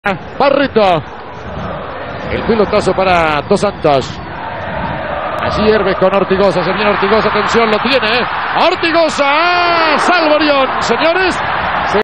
Boca 0 Cerro Porteño 0 Se lo pierde Ortigoza / 4tos Copa Sudamericana 2014
Cerro Porteño: Diego Barreto; Carlos Bonet, Víctor Hugo Mareco, Bruno Valdez, César Benítez; Mauricio Sperduti, Julio Dos Santos, Fidencio Oviedo, Óscar Romero; Dani Güiza y José María Ortigoza. DT: Leonardo Astrada.

Boca: Agustín Orion; Leandro Marín, Claudio Pérez, Juan Forlín, Nicolás Colazo; José Pedro Fuenzalida, Cristian Erbes, Fernando Gago, Marcelo Meli; Jonathan Calleri y Andrés Chávez. DT: Rodolfo Arruabarrena.

Árbitro: Martín Vázquez.
Estadio: Defensores del Chaco.